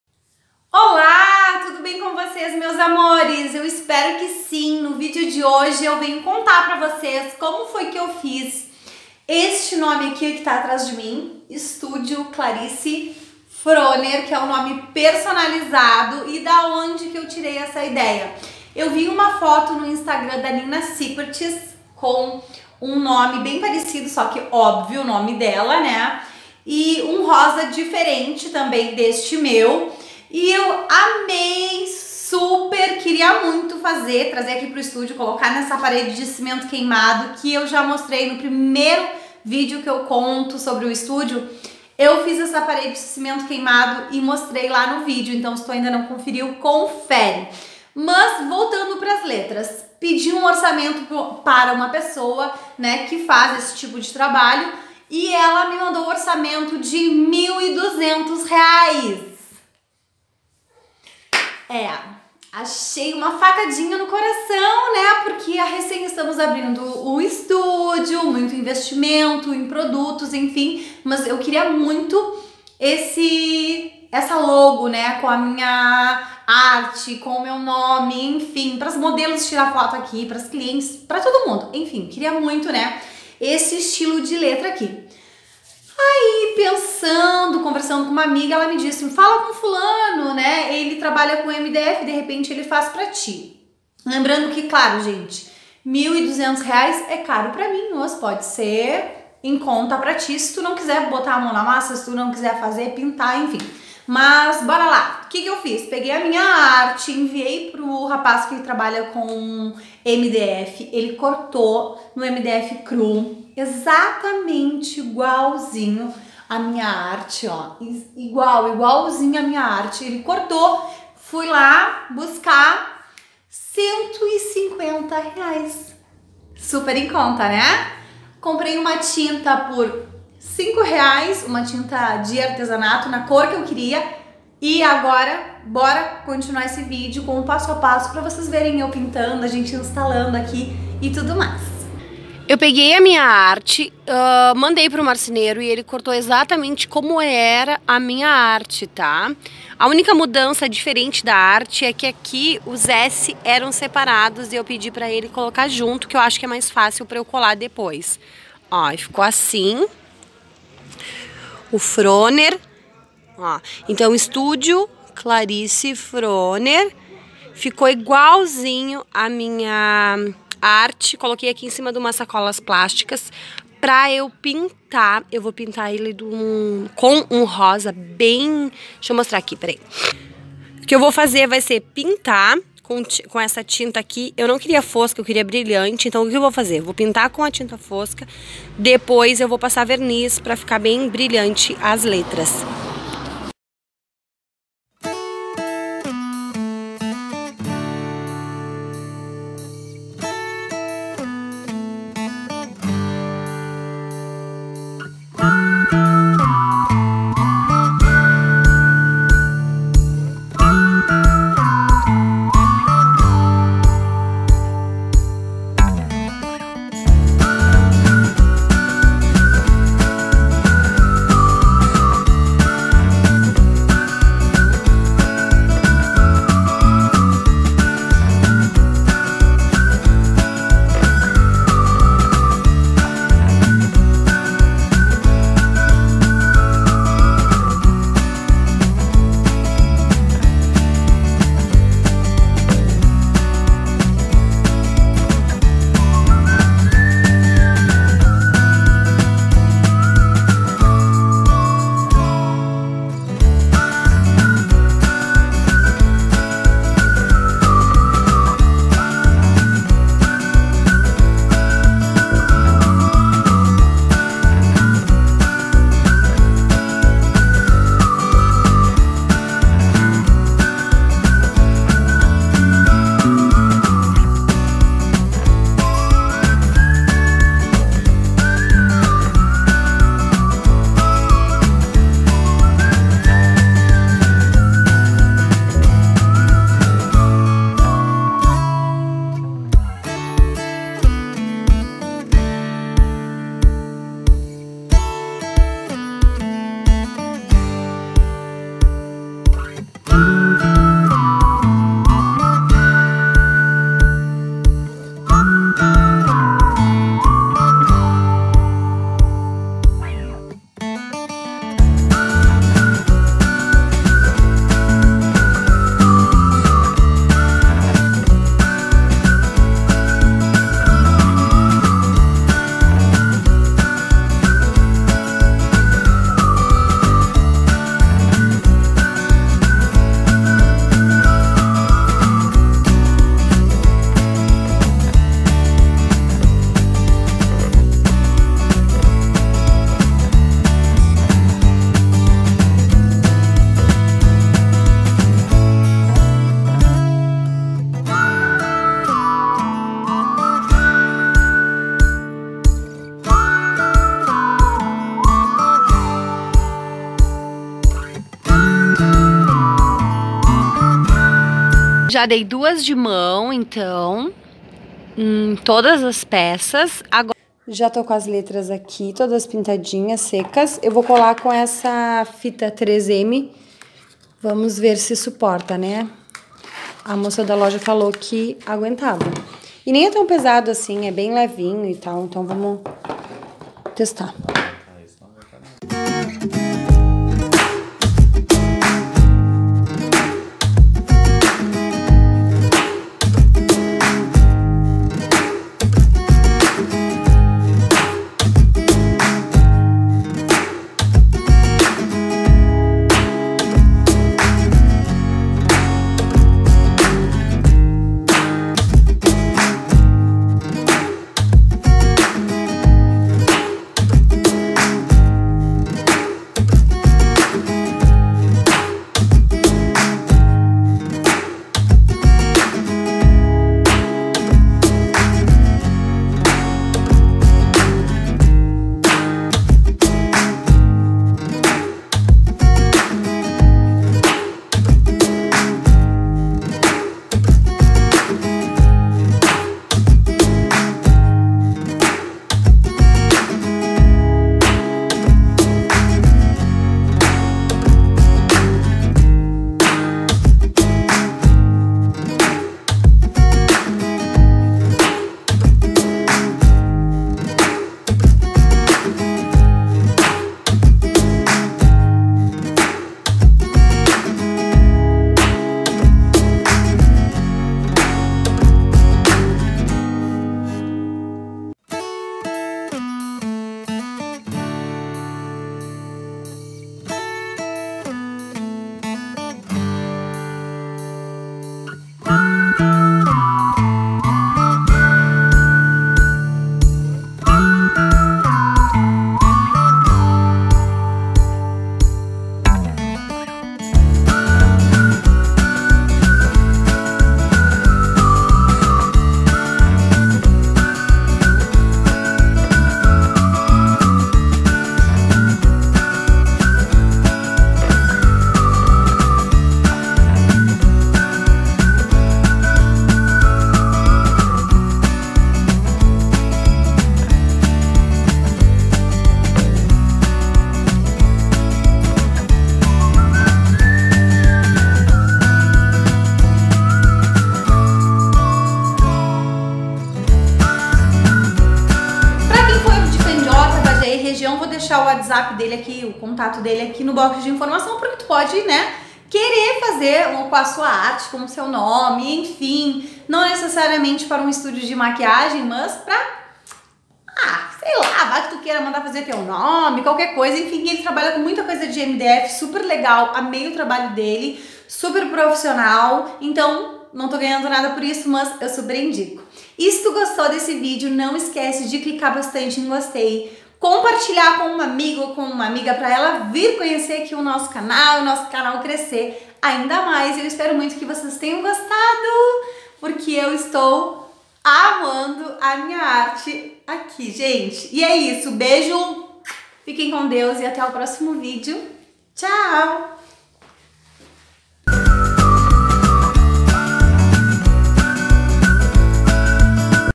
DIY NOME PERSONALIZADO DO STUDIO |Clarisse Froner
Olá! No vídeo de hoje vou mostrar o passo a passo de como reformar um armário de escola.
CONTATO DO MARCINEIRO: (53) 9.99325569

Vídeo que você pode gostar.